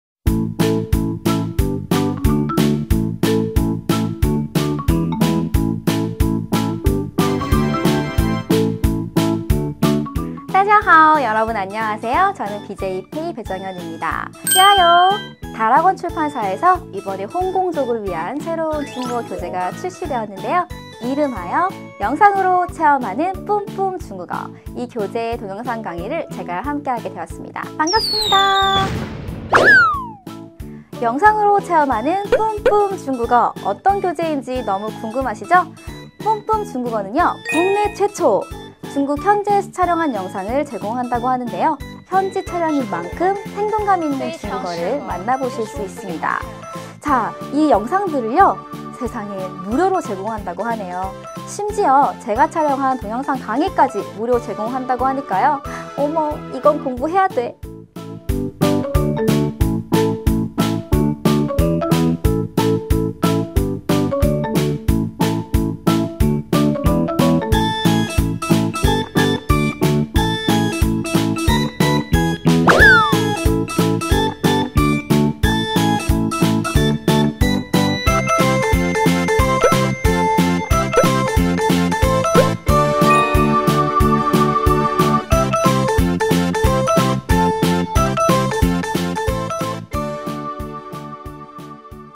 [북트레일러] 영상으로 체험하는 뿜뿜 중국어 탄탄대로 취업편
✔영어는 미드로 배우면서 중국어는 왜 어렵게 배워요?
✔이제 중국어도 영상 드라마로 배울 수 있어요!

100% 현지 촬영 영상 드라마로
리얼 중국어를 경험한다!

★영상으로 체험하는뿜뿜 중국어★
1. ‘두근두근 기초편’
2. ‘알콩달콩 일상편’
3. ‘탄탄대로 취업편’

1초 만에 중국어 뿜어내게 하는
“뿜뿜 중국어”

BJ 페이와 함께 이번엔 꼭 성공하세요!